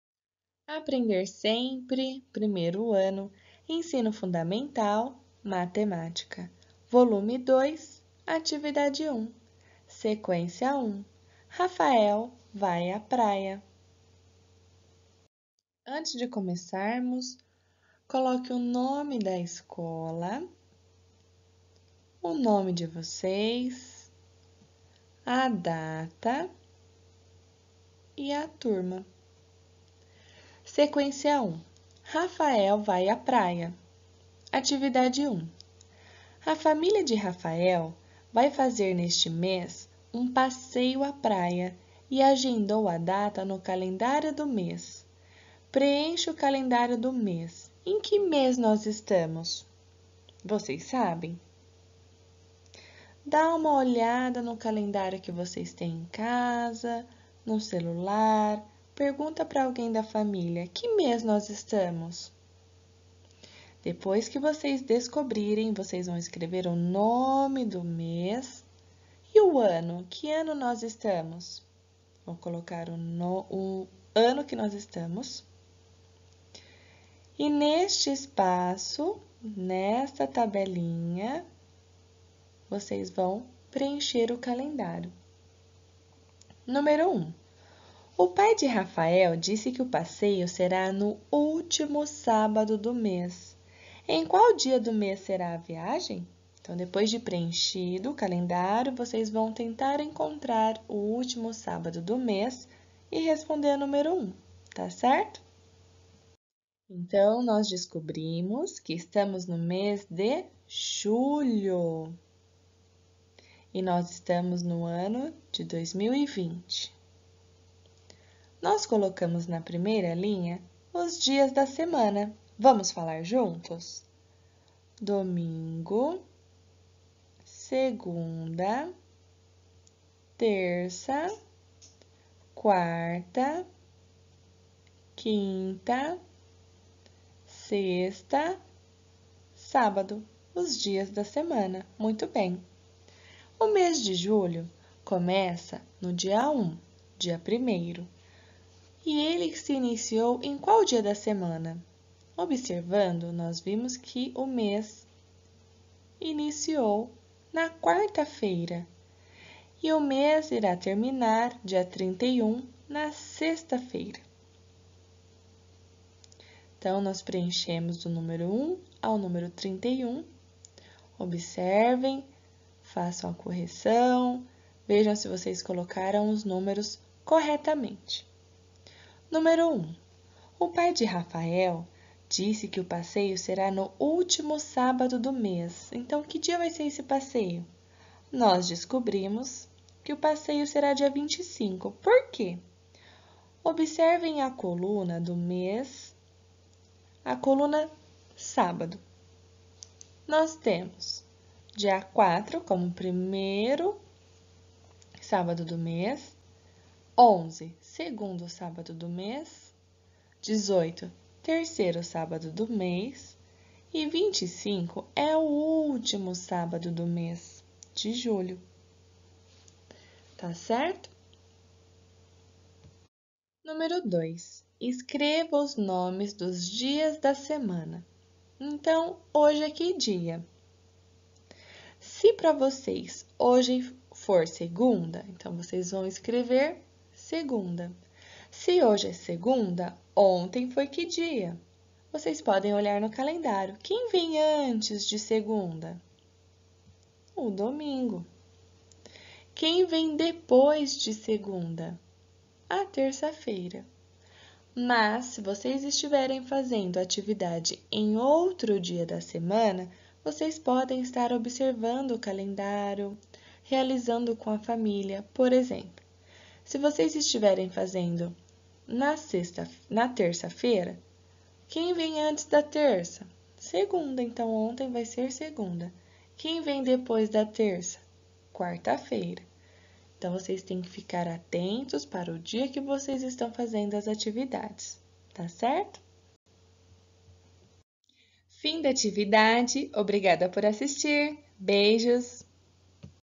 VOL. 2 APRENDER SEMPRE 1º ano atividade 1 (sequência 1 - RAFAEL VAI À PRAIA) Matemática
Vídeo de apoio para os alunos do ensino fundamental da Rede Estadual de Ensino do Estado de São Paulo.